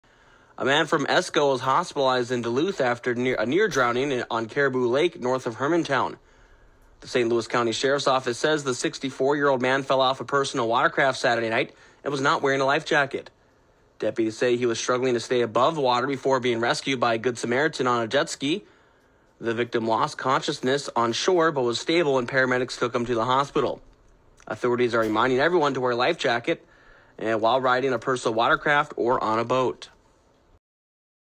Near Drowning In St. Louis County, MN
CARIBOU LAKE, MN (KROXAM.com) A near drowning on Caribou Lake near Hermantown, Minnesota.